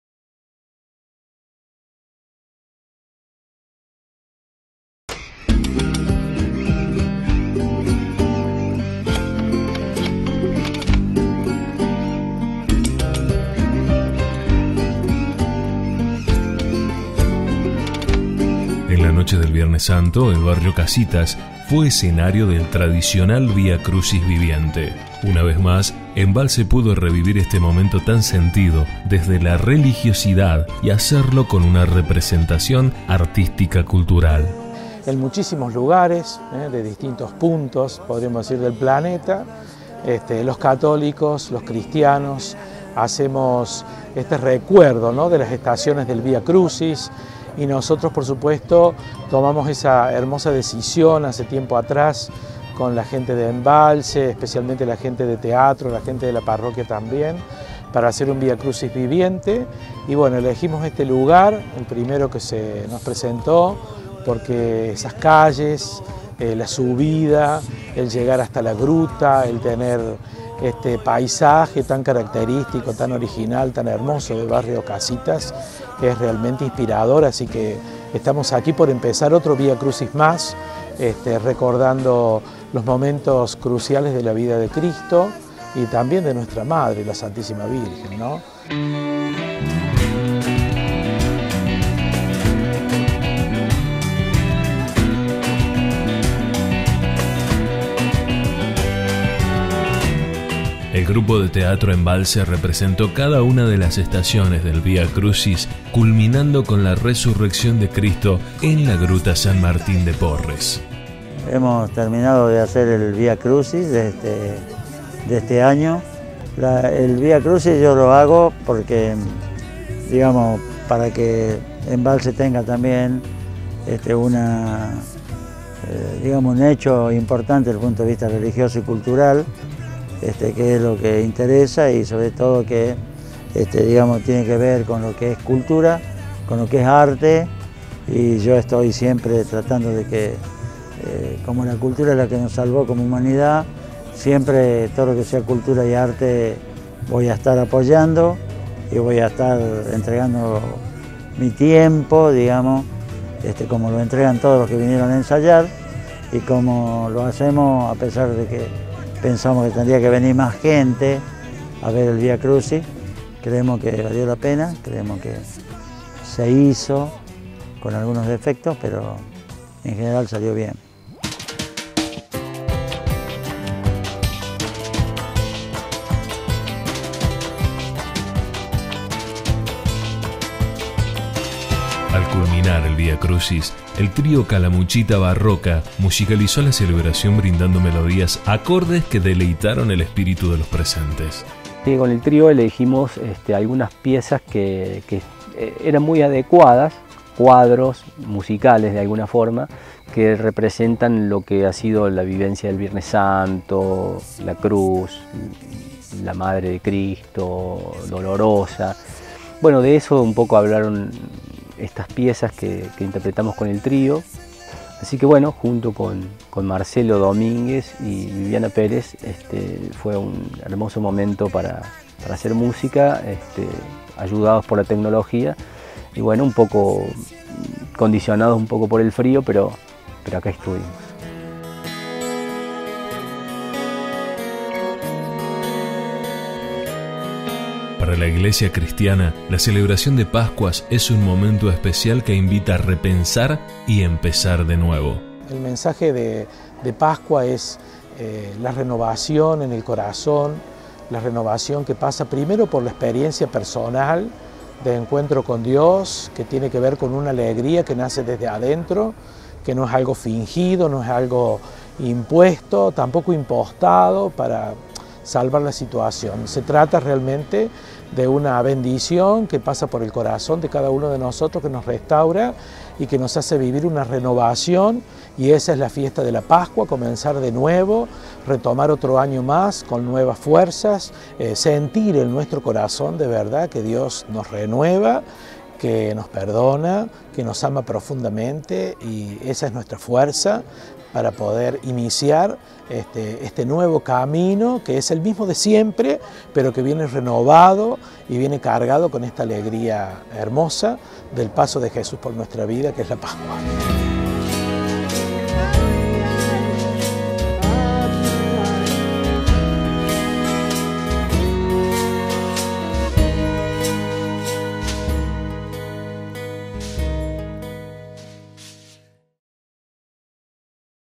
NOTA 1click - VIA CRUCIS EMBALSE 2016
En la noche del viernes santo el barrio Casitas, fue escenario del tradicional Vía Crucis viviente. Una vez más Embalse pudo revivir este momento tan sentido desde la religiosidad y hacerlo con una representación artística cultural.